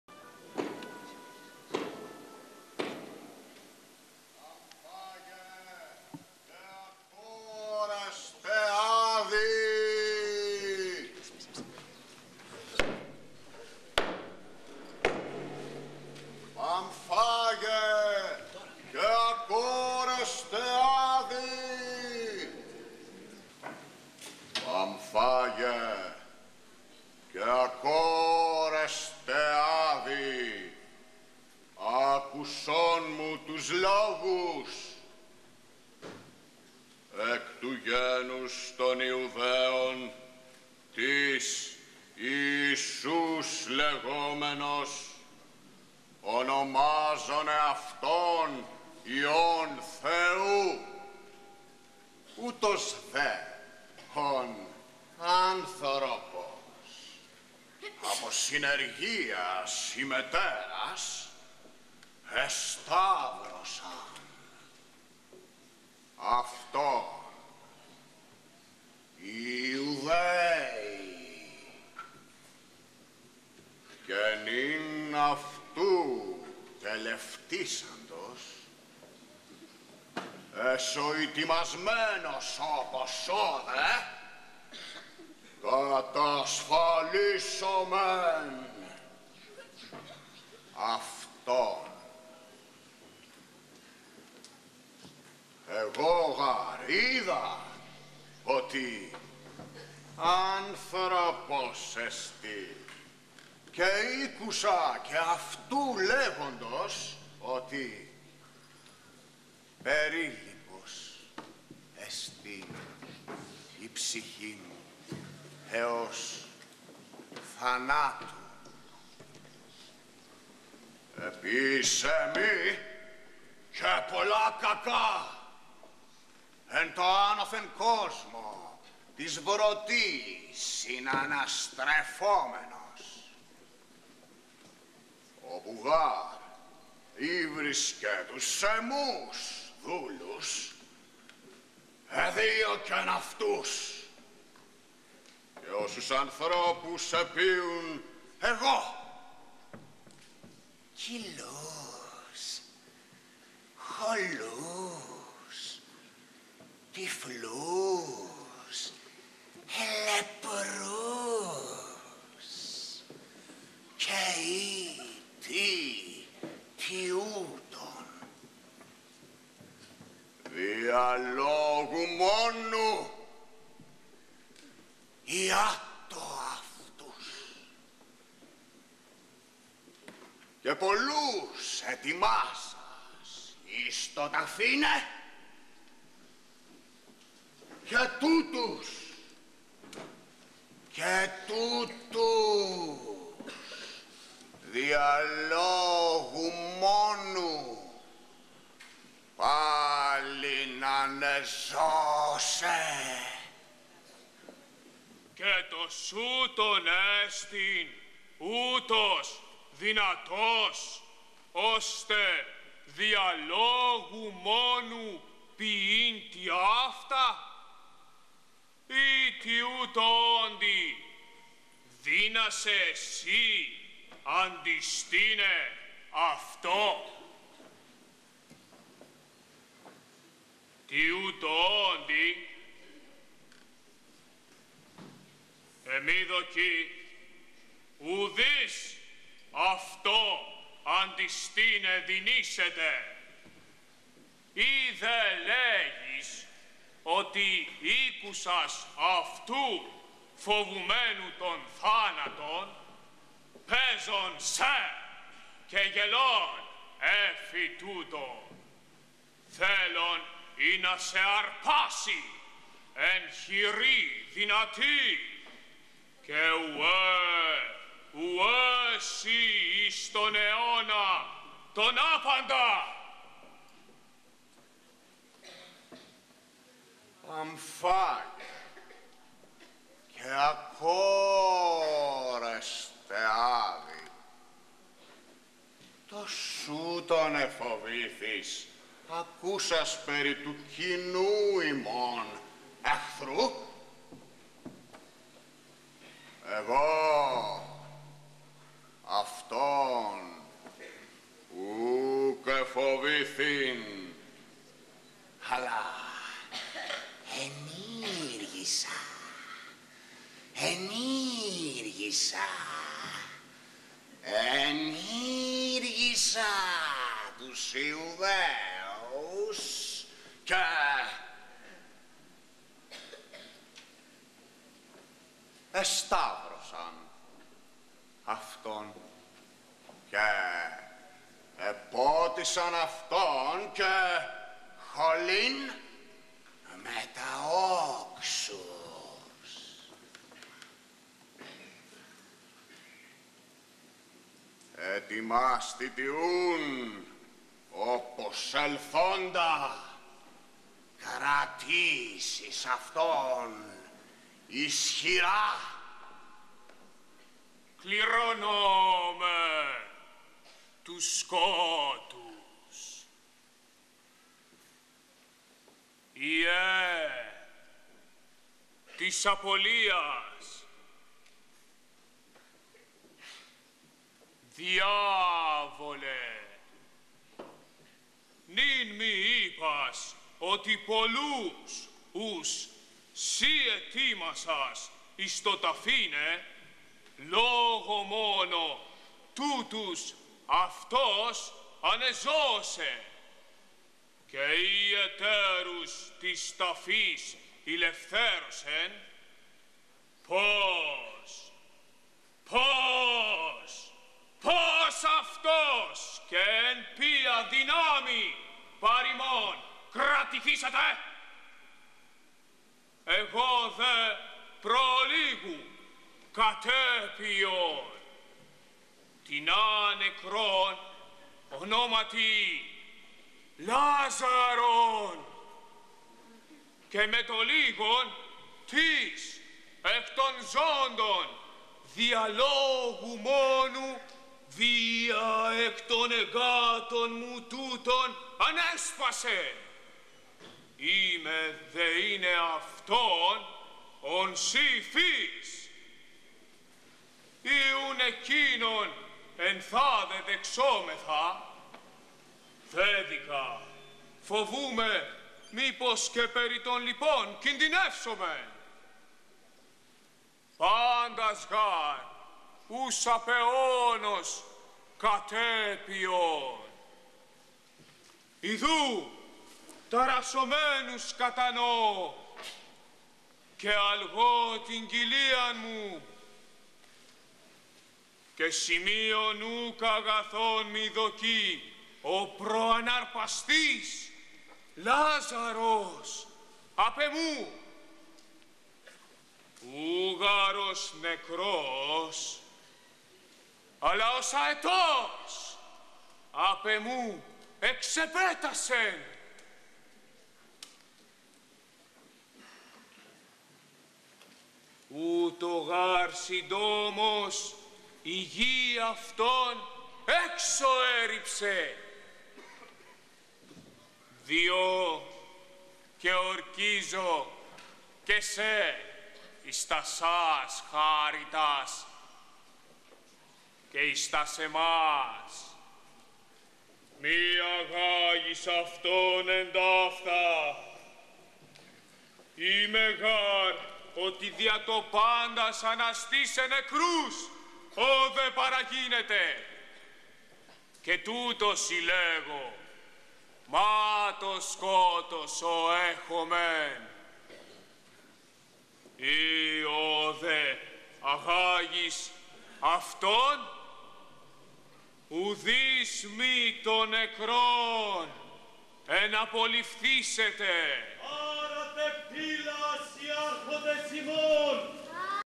ΠΑΣΧΑ - ΑΠΟΚΡΥΦΑ ΕΥΑΓΓΕΛΙΑ (Α) (original text) Theatre Erineos- Greece
/ΝΙΚΟΔΗΜΟΥ ΑΠΟΚΡΥΦΑ ΕΥΑΓΓΕΛΙΑ (Α) / (original text - πρωτότυπο κείμενο) Апокрифических евангелий / Σκηνοθεσία - Διδασκαλία: Ιωάννης Τράντας / Ερμηνεύουν: Άδης: Χαράλαμπος Μπάρδας - Σατάν: Γεώργιος Λεβέντης - Ψυχές: Εριφύλη Λεβέντη, Ελένη Μπάρδα - Άγγελοι: Παιδική Ομάδα του Λυκείου των Ελληνίδων Βόλου - Χοράρχης Αγγέλων: Ευάγγελος Κακάλιας - Δαυίδ: Ντομένικο Μπονάσσι - Ησαϊας: Παύλος Κιουρτσής / Από την παράσταση "Πάσχα των Ελλήνων" του "Λυκείου των Ελληνίδων Βόλου" σε συνεργασία με την Εστία Θεάτρου Βόλου Ερινεώς (3.4.04) / Teaching - Directed: Ioannis Trantas Estia Theatre Erineos Greece / Apocryphal Gospels / ملفق الانجيل Holly Easter / מפוקפק הבשורות